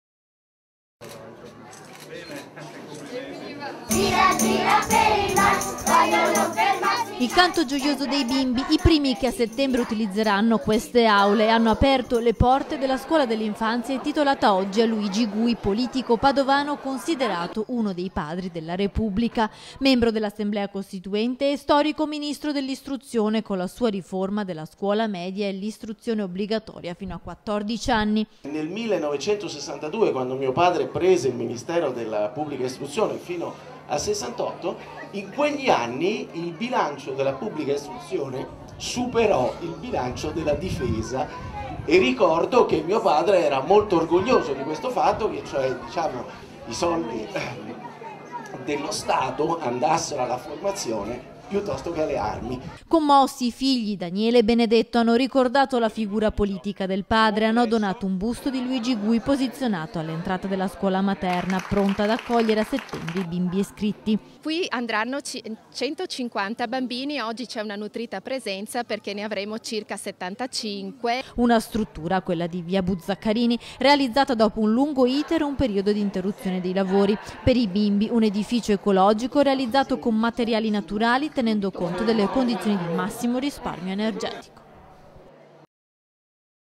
31/03/2015 - A PADOVA UNA SCUOLA MATERNA DEDICATA A LUIGI GUI
PADOVA TG (martedì 31 marzo 2015) - Grande festa oggi a Padova dov’è stata dedicata una scuola dell’infanzia alla figura storica di Luigi Gui il ministo padovano che approvò la riforma della scuola media negli anni sessanta…
Il canto gioioso dei bimbi, i primi che a settembre utilizzeranno le aule hanno aperto le porte della scuola dell’infanzia intitolata oggi a Luigi Gui, politico padovano considerato uno dei padri della Repubblica. Membro dell’assemblea costituente e storico ministro dell’Istruzione con la sua  riforma della scuola media e l’istruzione obbligatoria fino a 14 anni.
Commossi i figli Daniele e Benedetto hanno ricordato la figura politica del padre e hanno donato un busto di Luigi Gui posizionato all’entrata della scuola materna pronto ad accogliere a settembre i bimbi iscritti.

Una struttura quella di via Buzzacarini realizzata dopo un lungo iter e un periodo di interruzione dei lavori. Per i bambini un edificio ecologico realizzato con materiali naturali  tenendo conto delle condizioni di massimo risparmio energetico.